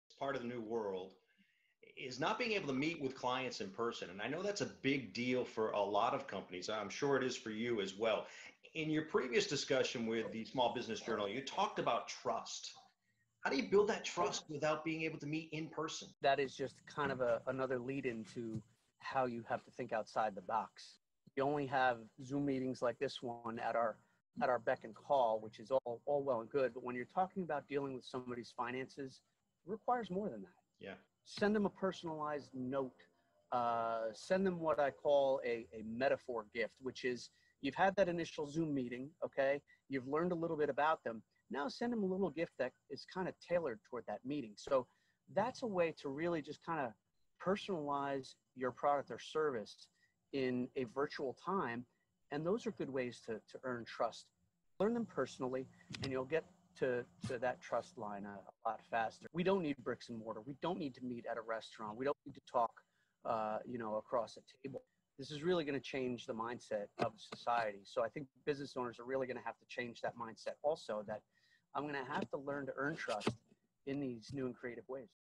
Christopher Kuhn ELO Consulting
Christopher founder ELO Consulting to help small to mid-sized businesses run more efficient financials. In this interview with the Small Business Journal we had the opportunity to chat with Christopher and hear what he's been doing to help entrepreneurs stay the course.